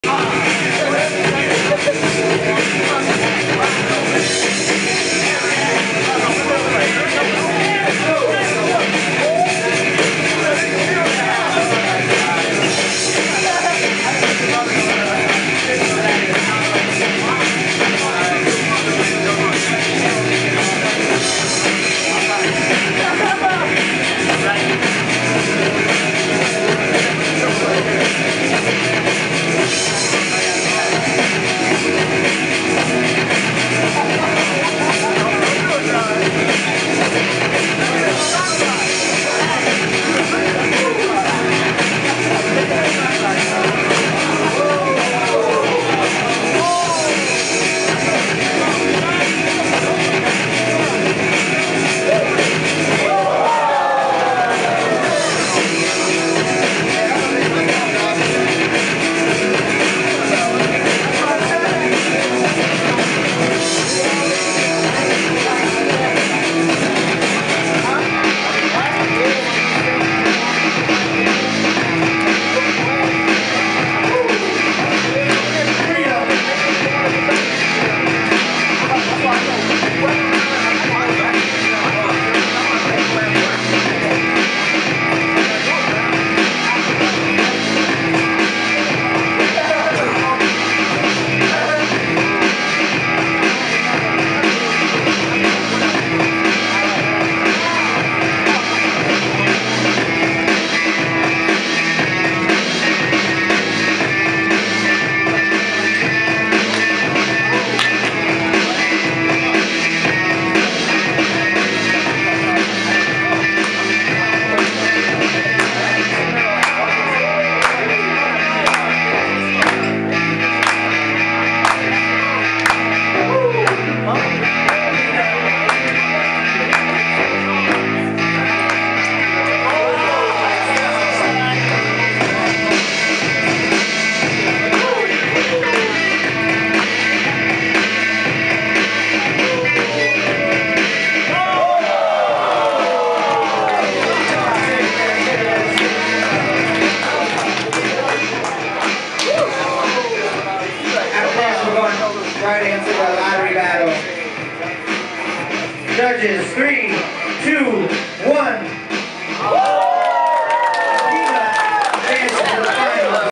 Top 4 - Peanut vs Freedom | 61Syx SDA 1 Year Anniversary Jam
1 vs. 1 - Top 4

Bboy Peanut | Self-X
vs.
Bboy Freedom | 99 Stylez Crew